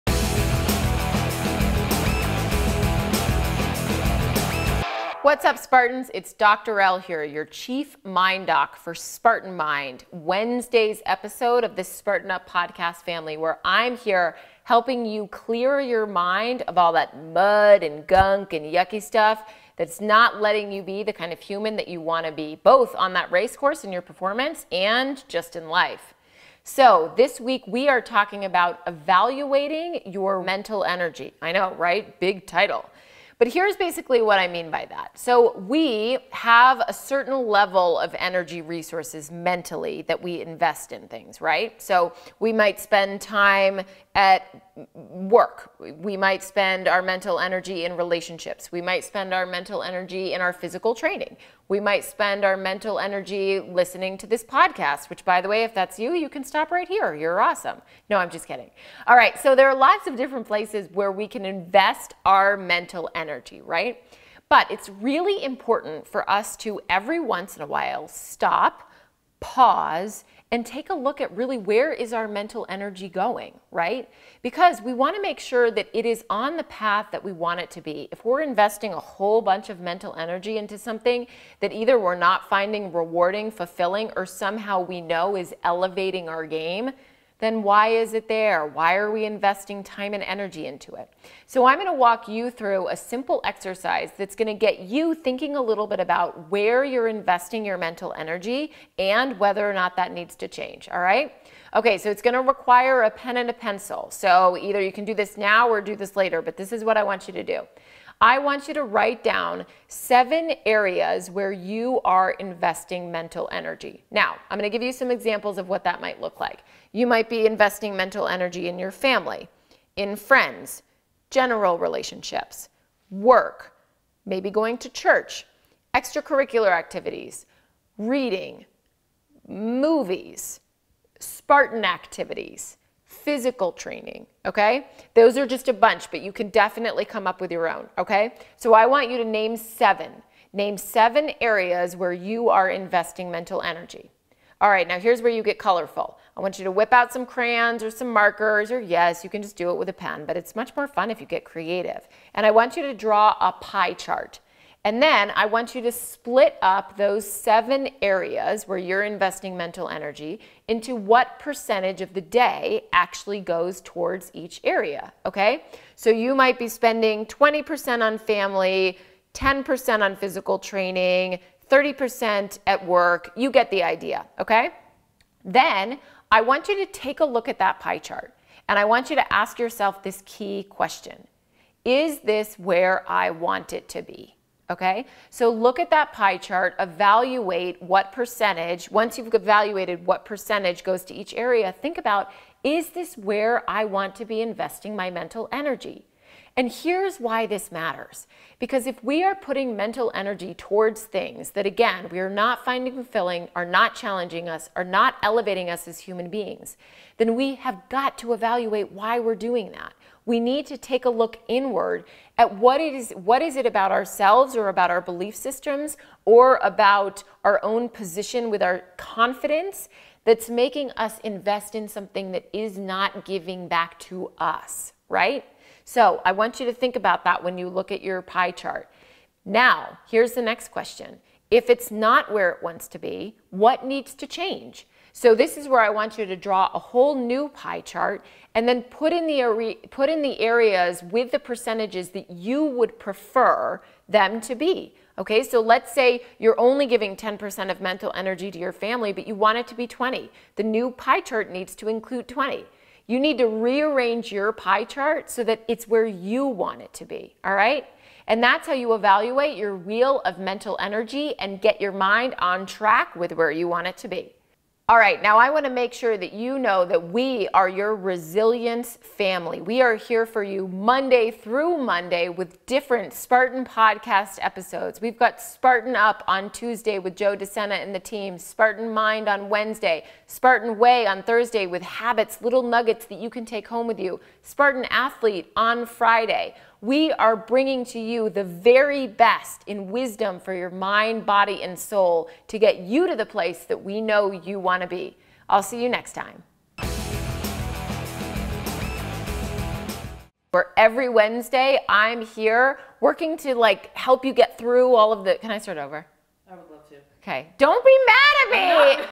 EVALUATING YOUR MENTAL ENERGY // SPARTAN MIND 042
We can only store and attend to so many things at any given time. Our mental energy actually has a limit. Because of this we need to be intentional on where we invest our mental energy and take inventory on what is occupying space in our mind. If we are more curious and intentional around where we are investing mental energy, we can adjust time spent so that very little time gets wasted. A simple exercise of evaluating your seven most filled areas can aid in getting you to where you truly want to be.

LESSONS:
Your mental energy is limited to being intentional with where you spend it can help create a more engaging life and leave you feeling more productive.
Try this simple activity to get your mental energy aligned with your intentions:
Make a pie chart and write down the 7 areas that take up most of the space in your head on a daily basis.
Identify what percentage each area takes up. For example, family may take up 10%, while work takes up 40%, and training 15%, and so on.
Ask yourself: Is this where I want it to be?
Finish with: What changes can I make to get it where I want to be?

CREDITS:

Production Team: Heather Knox, Knox Creative; Marion Abrams, Madmotion, llc.
Host: Dr. Lara Pence
Synopsis: Dr. Lara Pence